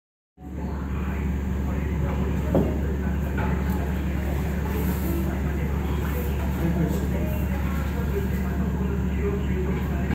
塩尻 信州 五十六家 ラーメン
中盛りチャーシュー　海苔の香りがgood
吉田家姉妹店